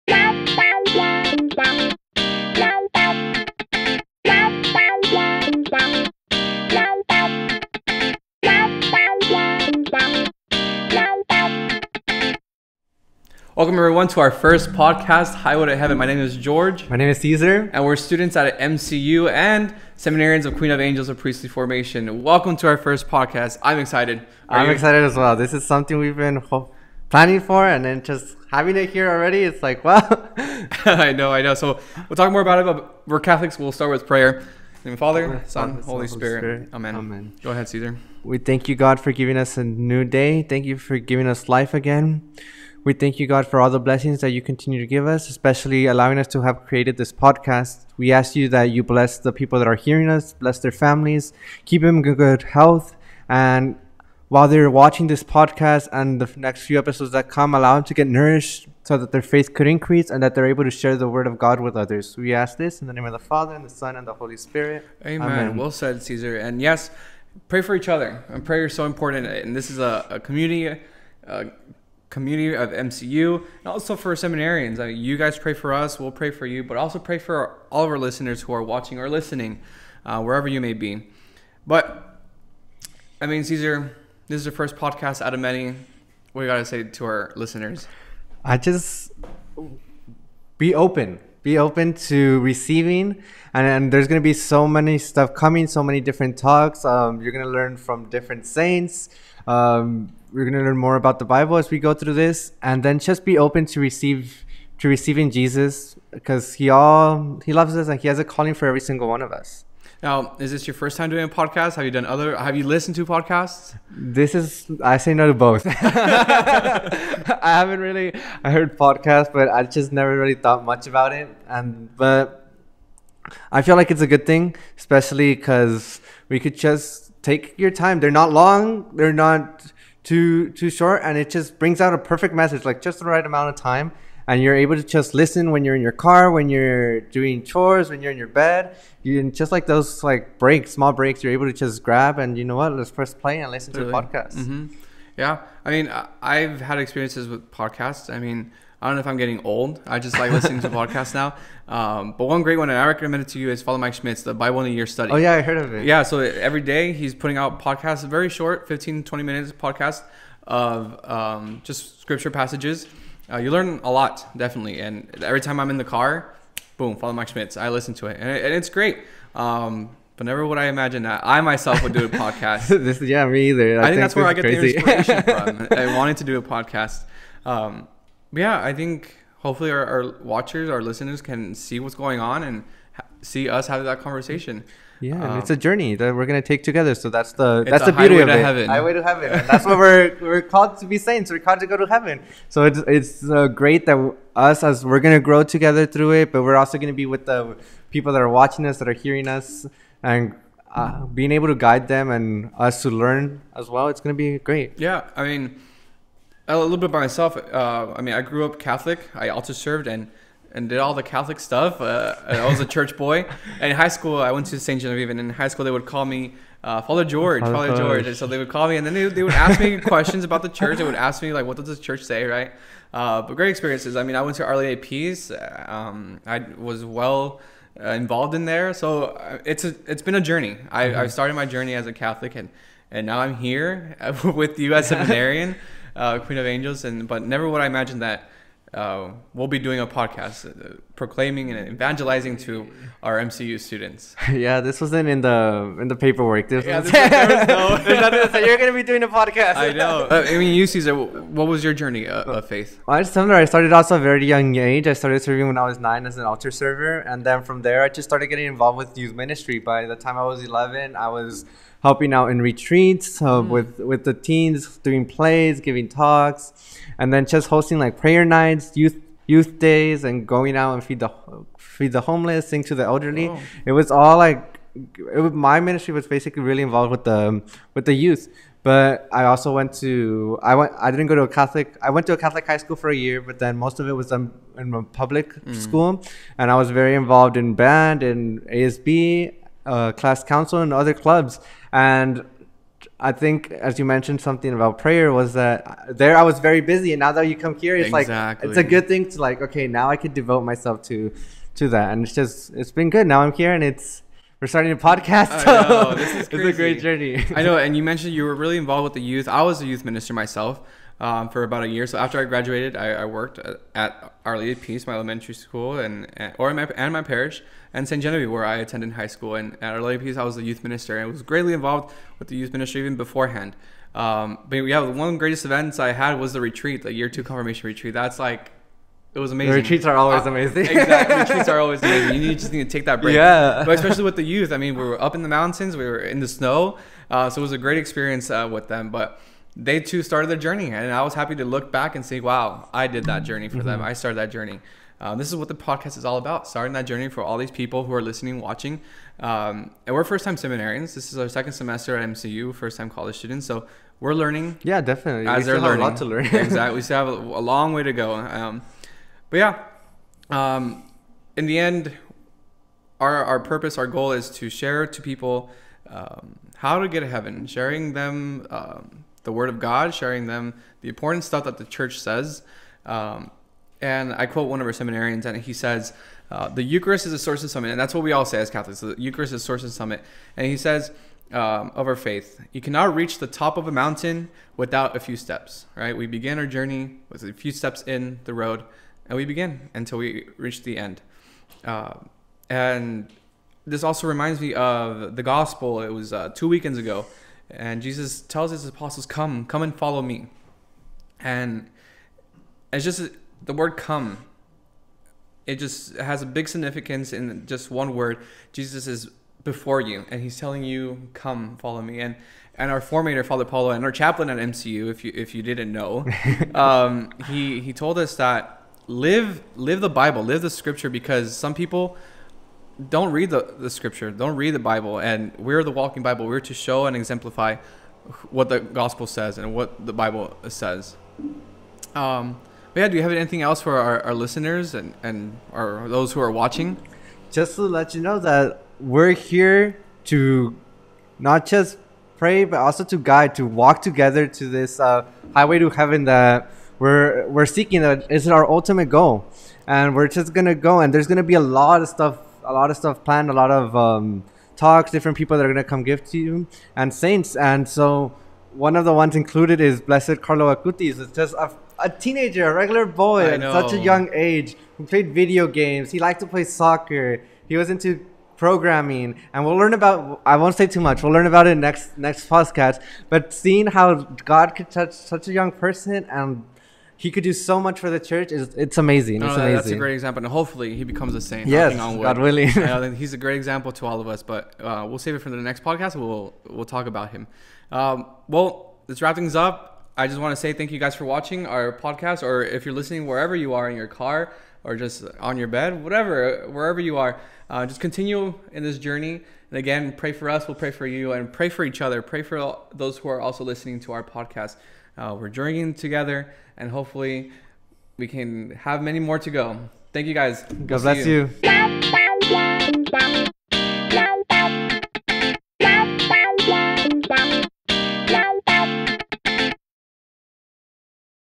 Highway to Heaven Podcast: Every Journey Has a Beginning
Marymount California University's Campus Ministry introduces the Highway to Heaven podcast hosted by student seminarians George Gomez and Cesar Sepulveda. In this first edition, "Every Journey Has a Beginning," the hosts share their stories and what interested them to pursue a priestly vocation.